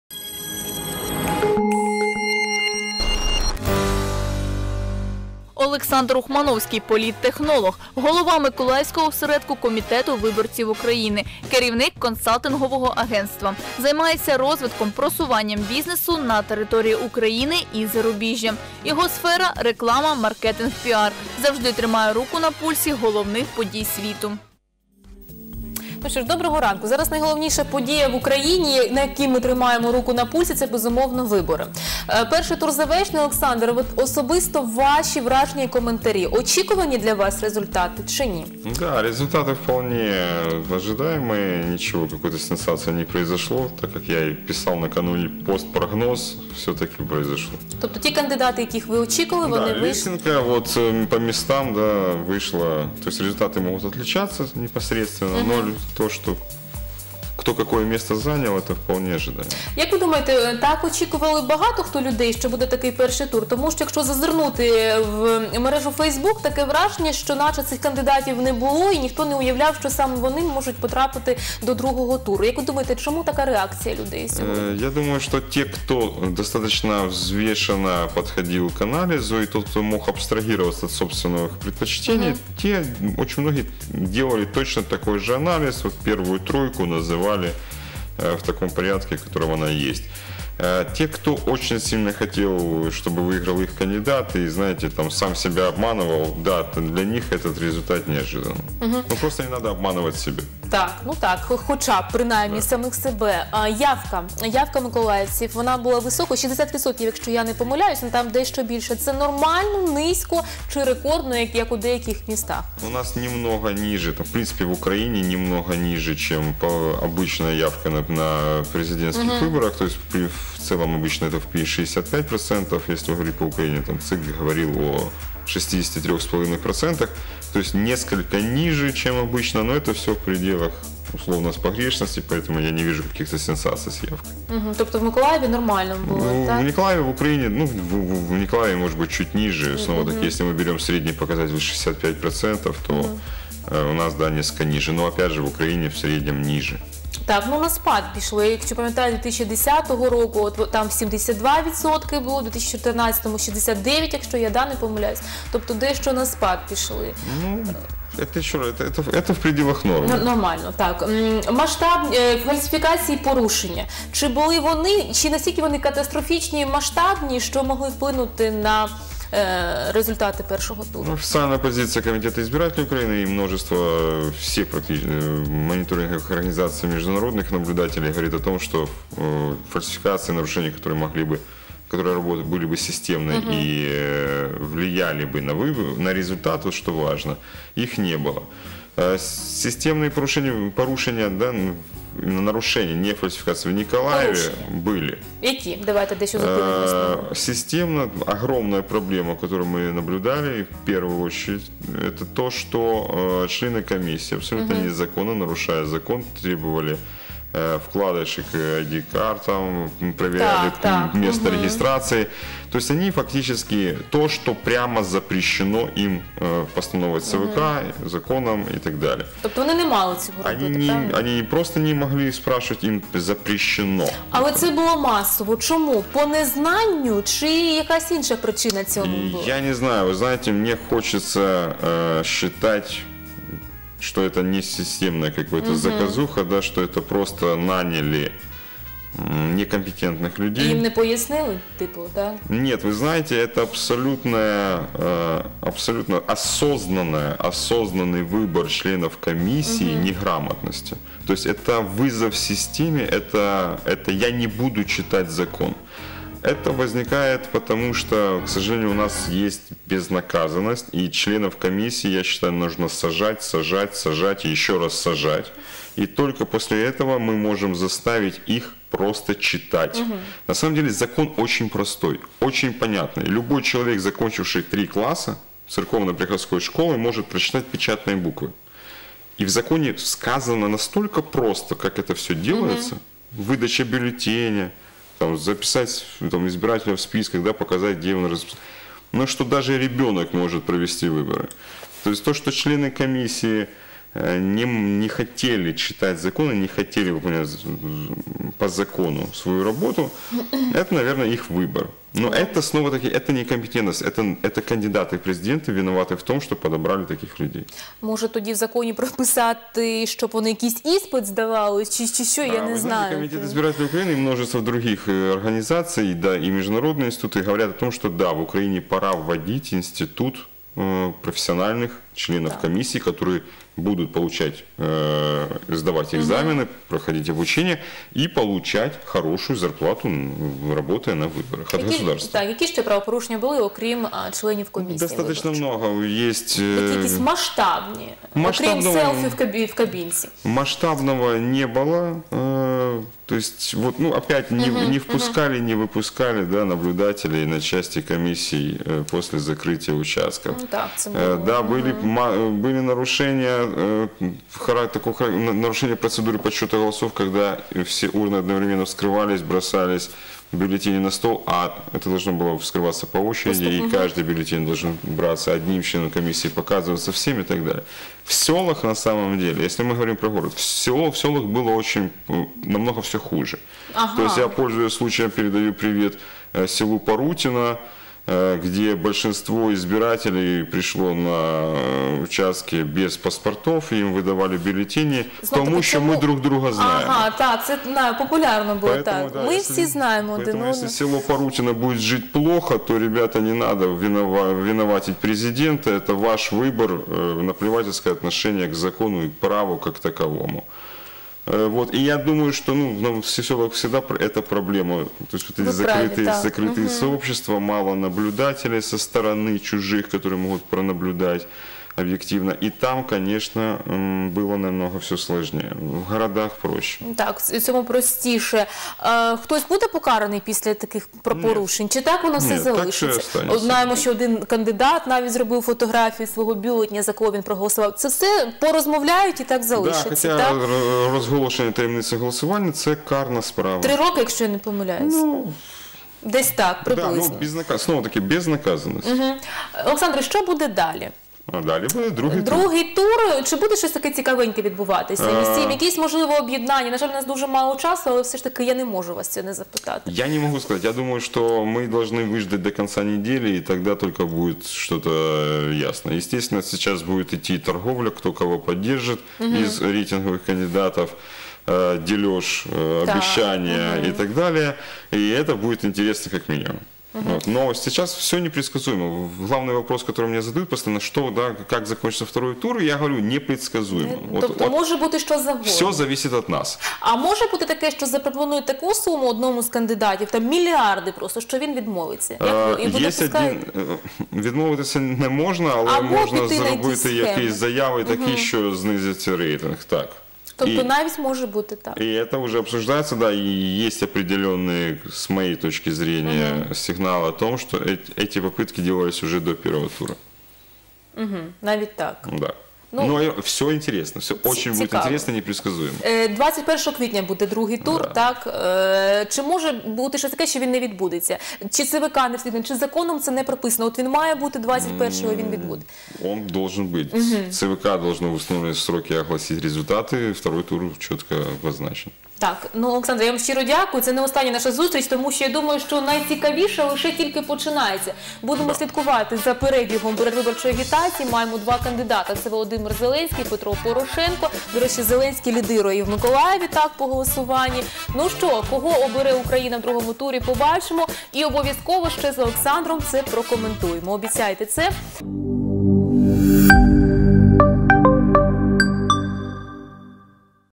02.04.2019. Новий день. Підсумки голосування в Миколаївській області
Важливе  для майбутнього країни голосування відбулося.
Наймасовіші порушення під час виборів по Миколаївській області, за словами політтехнолога Олександра Ухмановського, були зафіксовані у селах: виборці приходили на дільниці без паспортів.
Які результати президентських перегонів на даний момент? Чого чекати від другого туру виборів? Та чому компетенція членів виборчих комісій викликає сумніви?

Гість: Олександр Ухмановський, політтехнолог
Ведуча: Ірина Федоровська